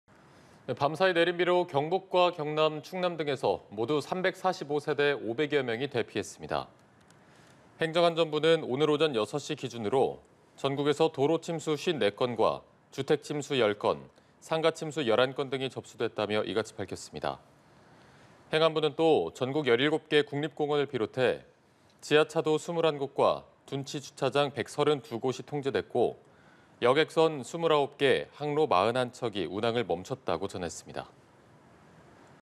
집중호우에 전국서 500여명 대피…도로 침수 54건 / 연합뉴스TV (YonhapnewsTV)
집중호우에 전국서 500여명 대피…도로 침수 54건

밤사이 내린 비로 경북과 경남, 충남 등에서 모두 345세대 500여명이 대피했습니다.

행정안전부는 오늘(21일) 오전 6시 기준으로 전국에서 도로 침수 54건과 주택 침수 10건, 상가 침수 11건 등이 접수됐다며 이같이 밝혔습니다.

행안부는 또 전국 17개 국립공원을 비롯해 지하차도 21곳과 둔치주차장 132곳이 통제됐고, 여객선 29개 항로 41척이 운항을 멈췄다고 전했습니다.